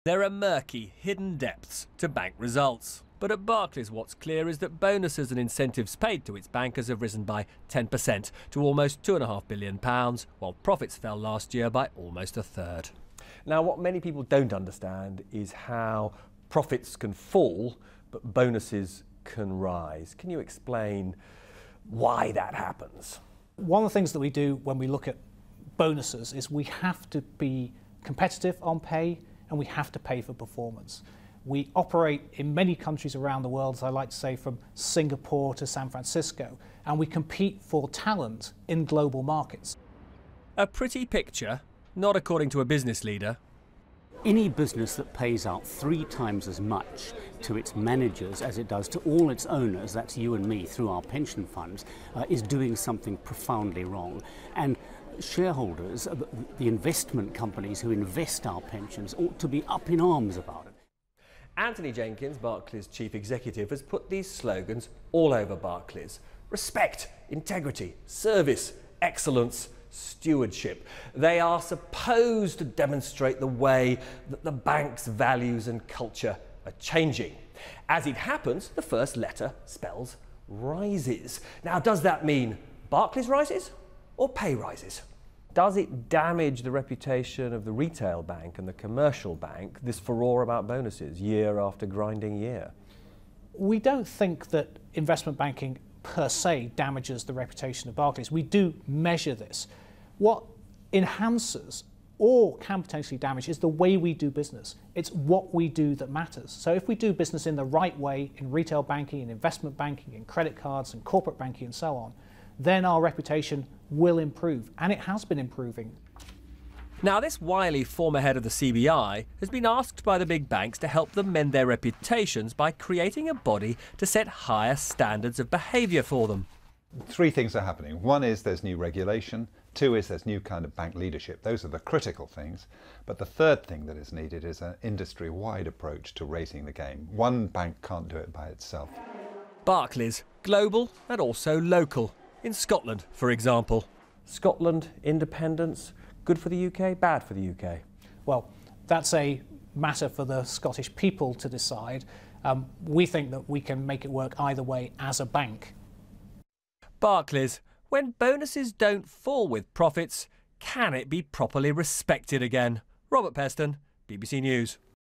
Barclays increases staff bonuses
Barclays increases staff bonuses Barclays increases staff bonuses Barclays increases staff bonuses  Barclays increases staff bonuses despite fall in profits Barclays has increased its staff bonuses despite a fall in profits.